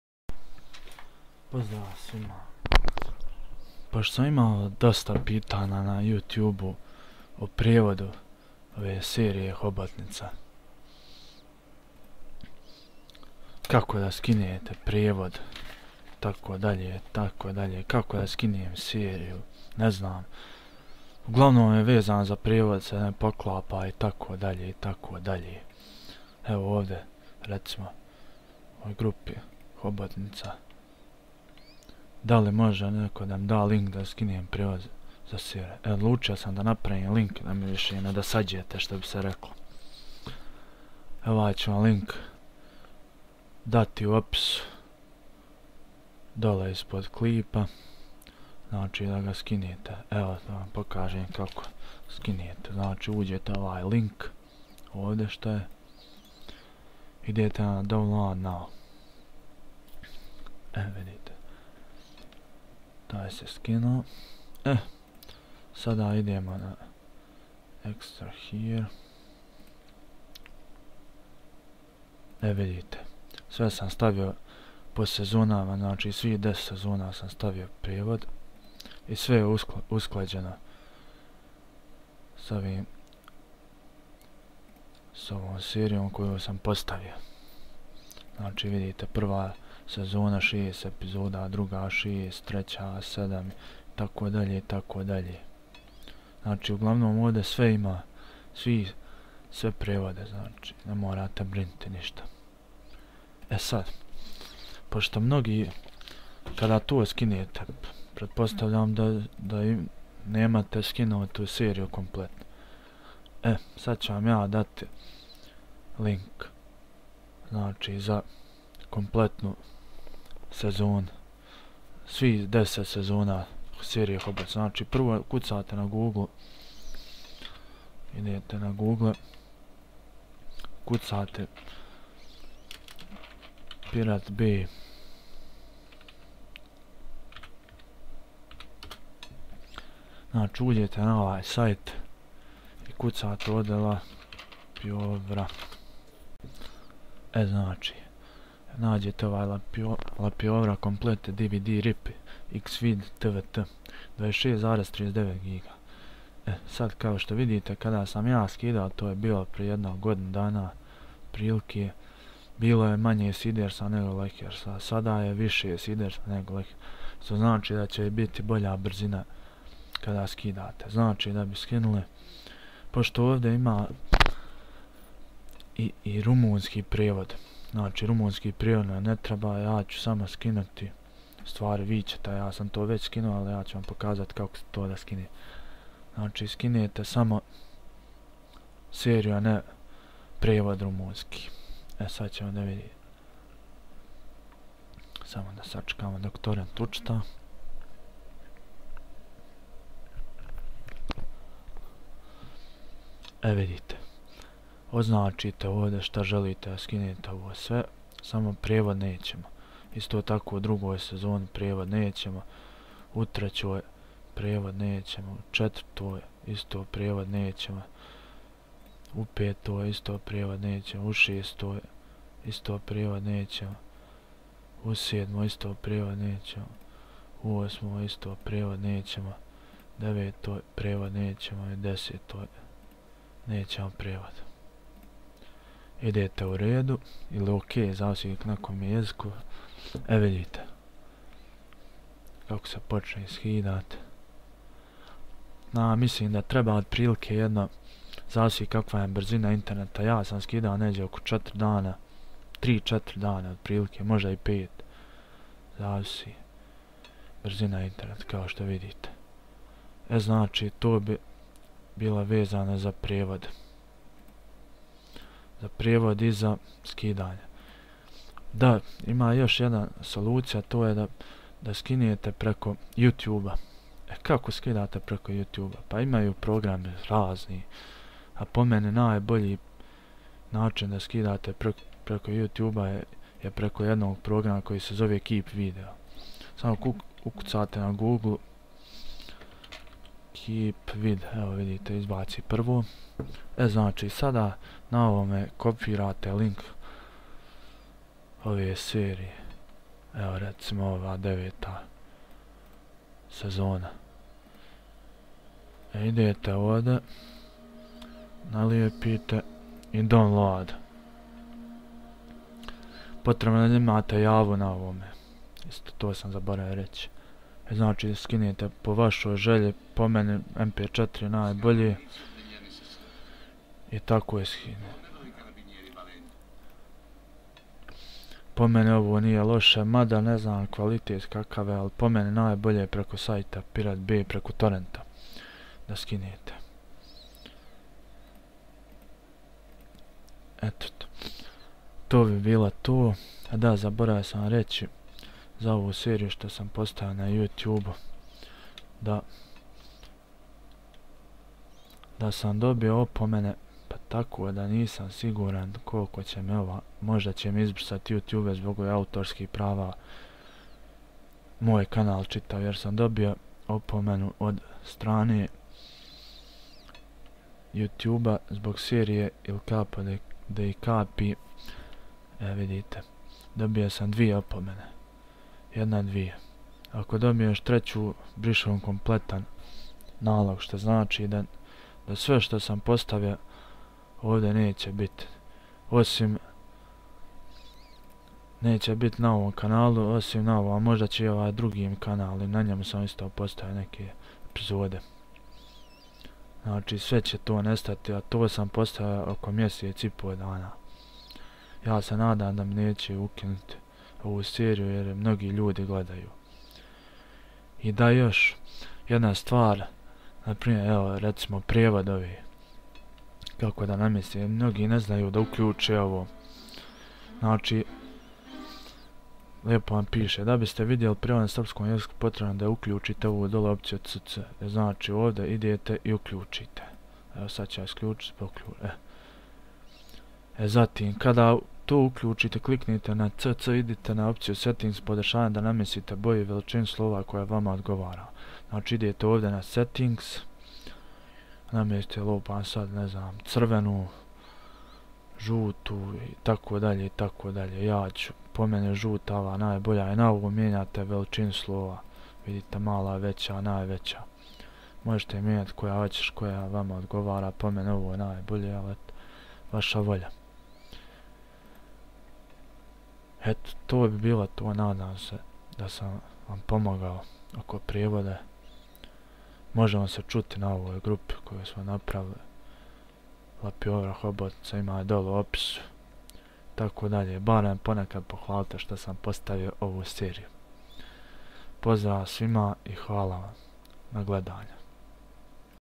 HOBOTNICA KOMPLETNA SERIJA I SRPSKI PREVOD [DOWNLOAD LINK]
KAKO SKINUTI PREVOD ZA ITALIJANSKU SERIJU HOBOTNICA I KAKO SKINUTI CIJELU SERIJU.DOWNLOAD LINK ZA PREVOD LINK ZA TORENT